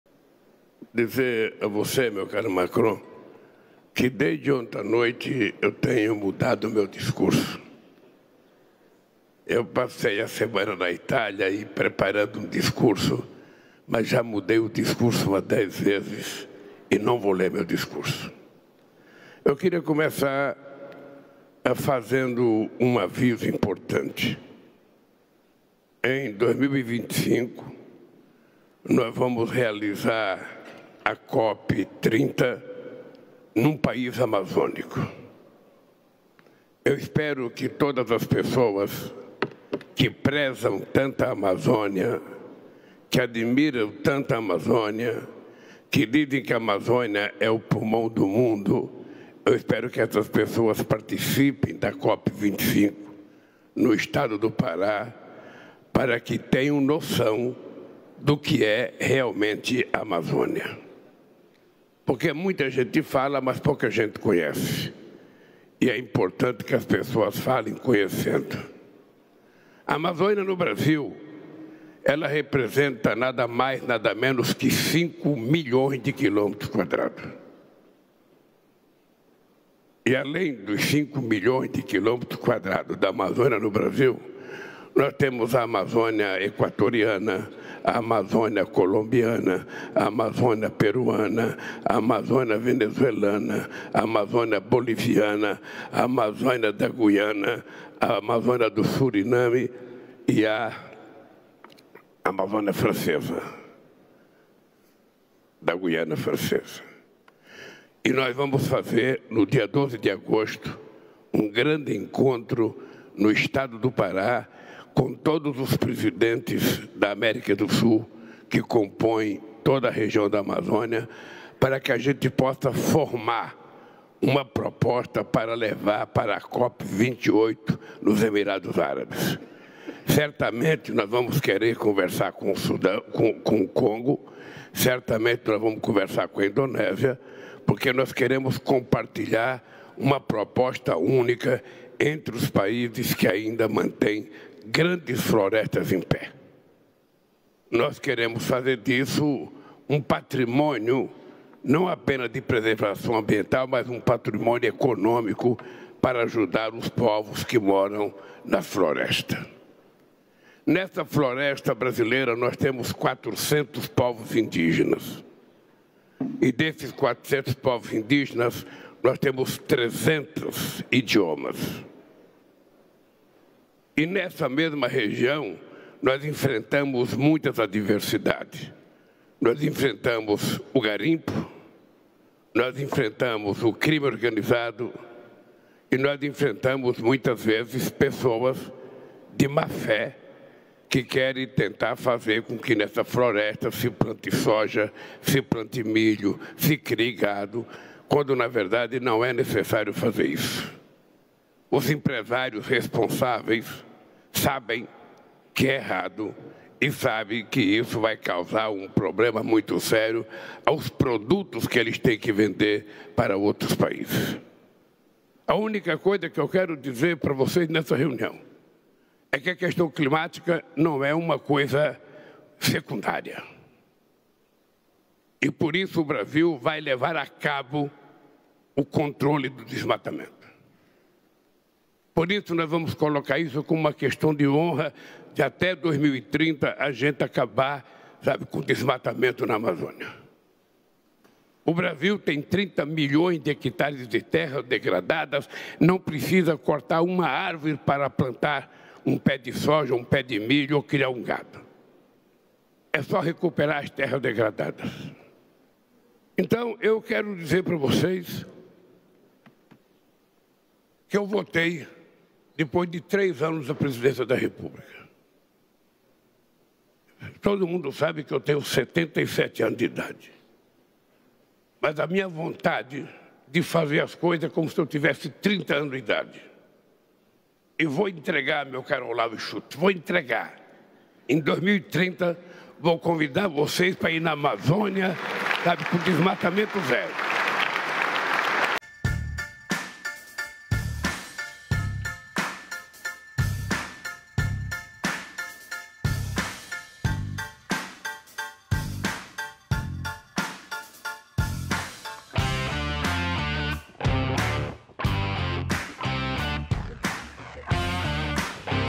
'EM 2030, VOU CONVIDAR VOCÊS PARA IR À AMAZÔNIA COM DESMATAMENTO ZERO', DIZ LULA
O presidente Luiz Inácio Lula da Silva (PT) discursou, nesta sexta-feira (23), na Cúpula do Novo Pacto de Financiamento Global, promovida pelo presidente francês, Emmanuel Macron, em Paris. O chefe do Executivo foi aplaudido pelas lideranças políticas presentes ao dizer que os “convidará” para ir na Amazônia com desmatamento zero em 2030, lembrando que essa foi uma de suas promessas de campanha nas eleições presidenciais do ano passado.

“Nós estamos caminhando para cumprir a nossa famosa proposta de campanha que é chegar ao desmatamento zero em 2030”, disse.